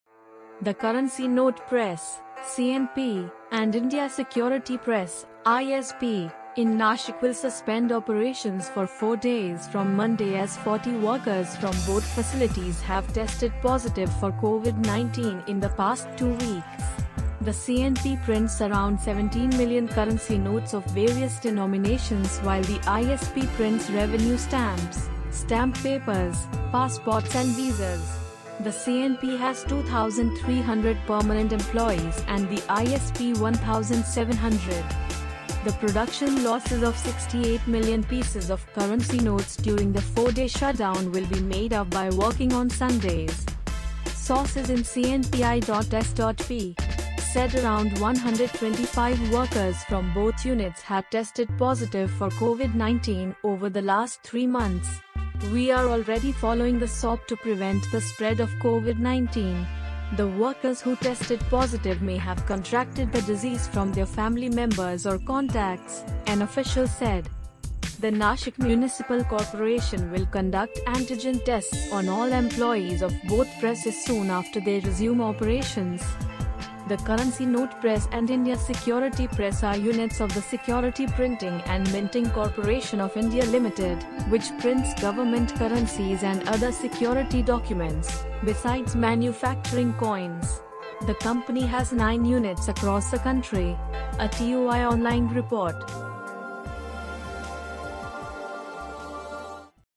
Nashik: Money press shut for 4 days after 40 staffers test Covid positive
The Currency Note Press and India Security Press in Nashik will suspend operations as 40 workers from both facilities tested positive for Covid-19. The operations will be suspended for 4 days. The CNP prints around 17 million currency notes of various denominations while the ISP prints revenue stamps, stamp papers, passports and visas. The CNP. has 2,300 permanent employees and the ISP 1,700. Sources in CNP-ISP said around 125 workers from both units had tested positive for Covid-19 over the last three months. “We are already following the SOP to prevent the spread of Covid-19. The workers who tested positive may have contracted the disease from their family members or contacts,” an official said.